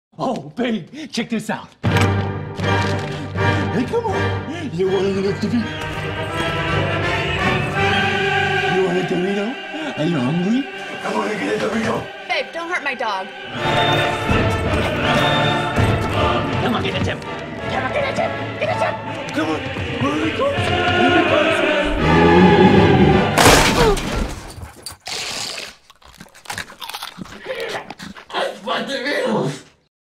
Собака реклама чипсов.
Один из многих рекламных роликов с участием собак,в нашей подборке "собаки в рекламе".